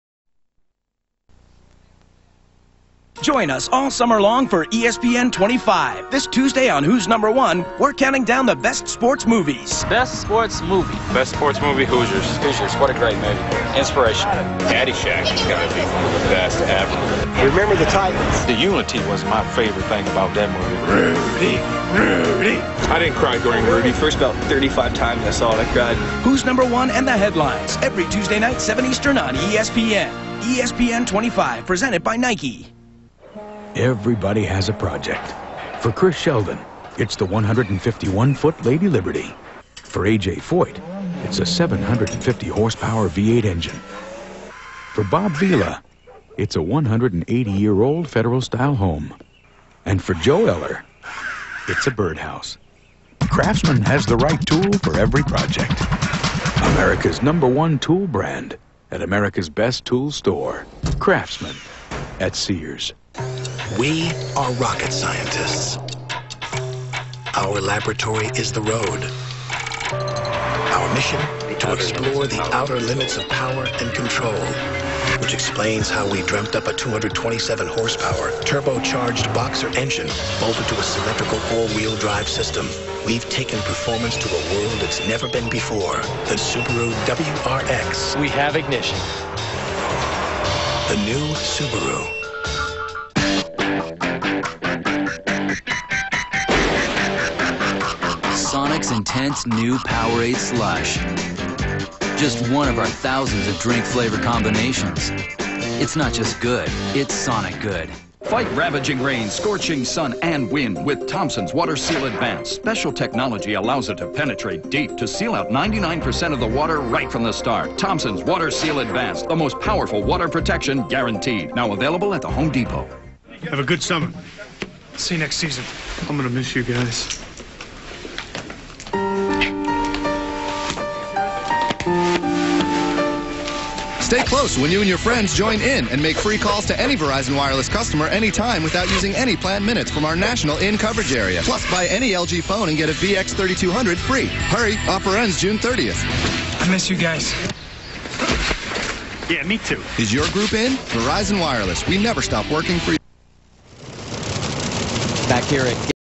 6/27/2004 ESPN2 Commercials Part 11
Commercials aired during the 2004 Sears Craftsman NHRA Nationals.
-ESPN25 promo
-Craftsman (A.J. Foyt and Bob Vila)
-Subaru WRX (Lance Armstrong)
-Sonic
-Thompson's Water Seal Advanced
-Verizon Wireless